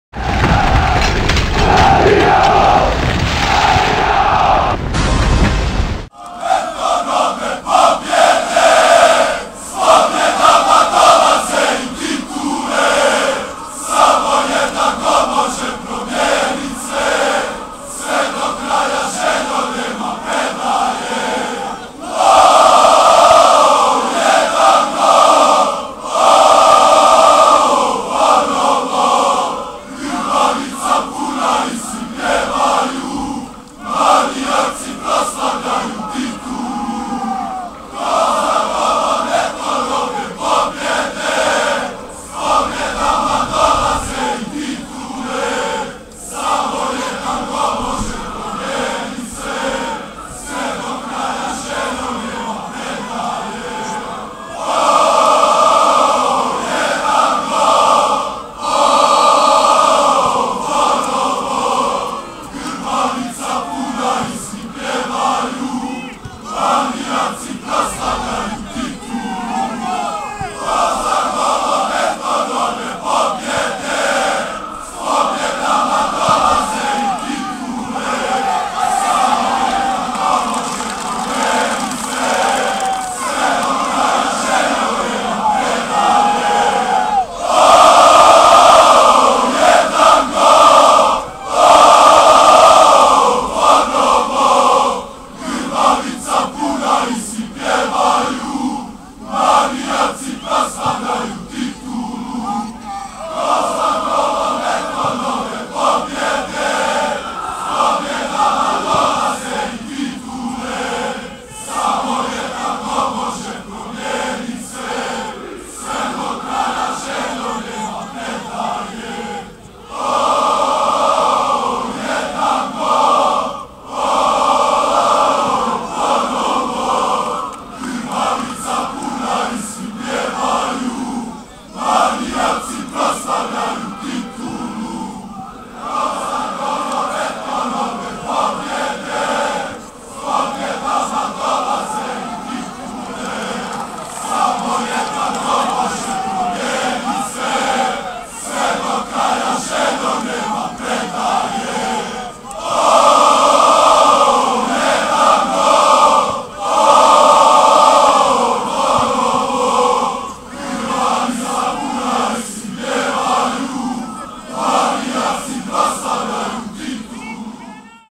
Slavija - Željeznicar 1:2 TM87 - Gol za golom
20. kolo / Lukavica, FK Slavija - FK ŽELJEZNIČAR / 7.4.2013

Gol za golom eto nove pobjede...